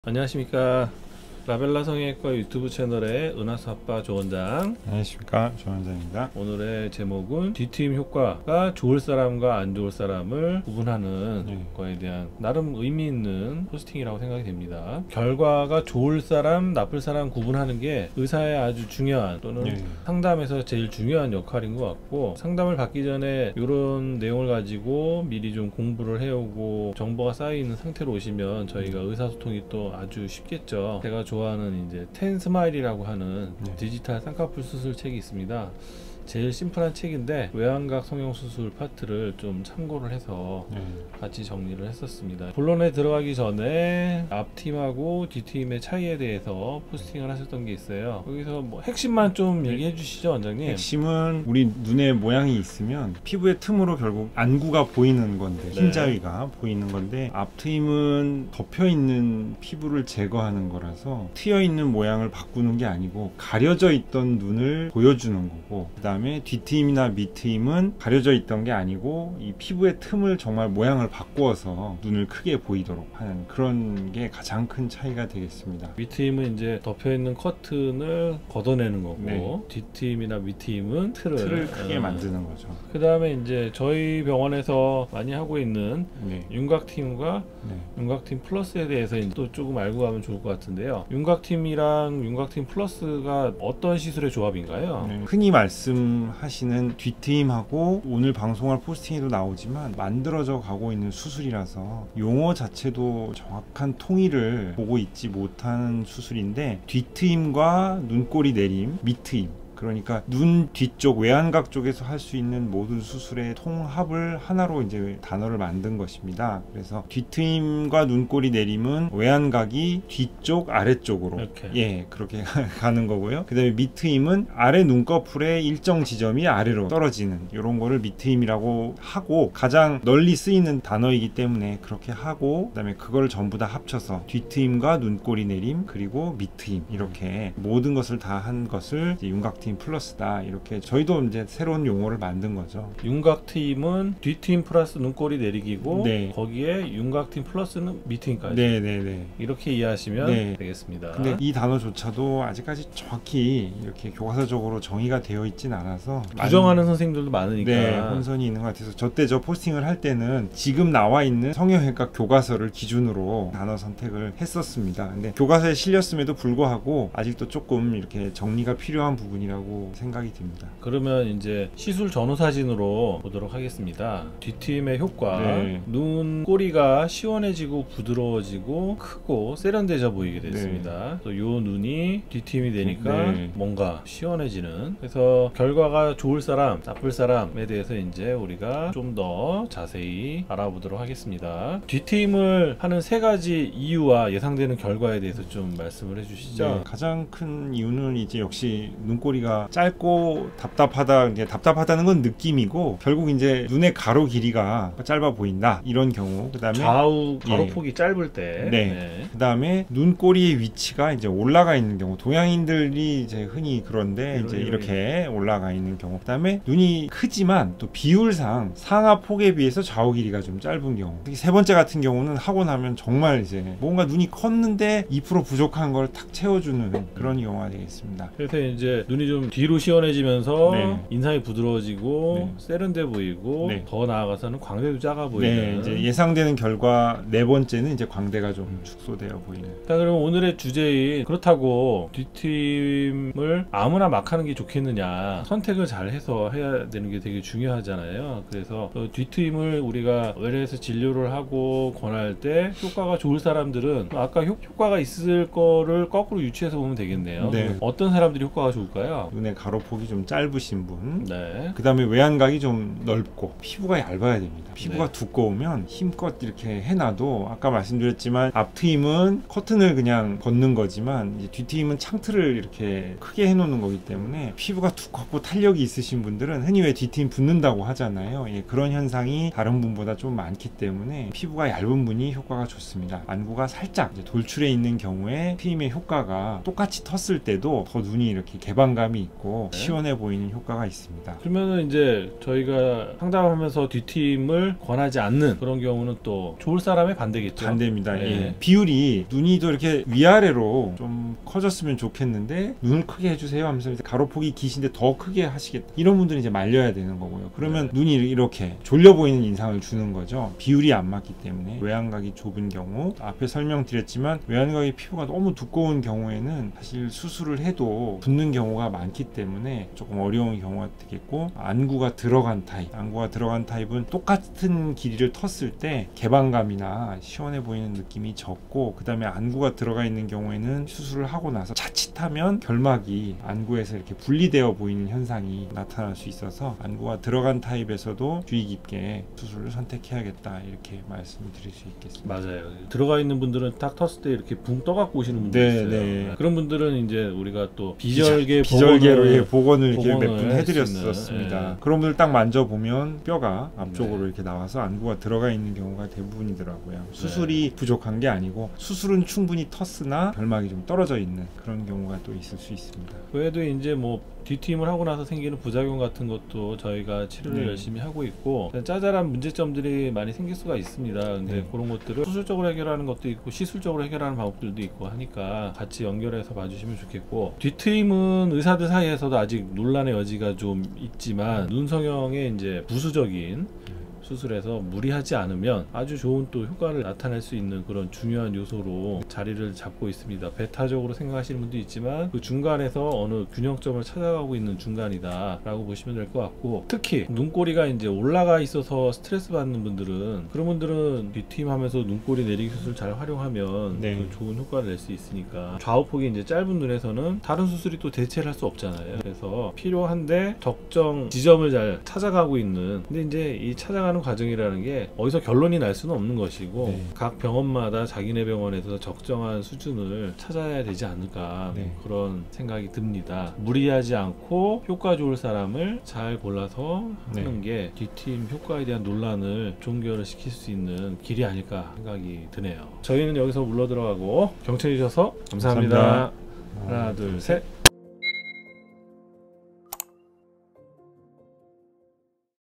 눈 뒤트임 받으면 효과 좋을 사람과 안 좋을 사람 알려드립니다 | 앞트임 뒤트임 밑트임
라벨라 따봉21 블로그

진료 상담은 라벨라 카카오톡으로 사진 문의 부탁드립니다

라벨라 원장 올림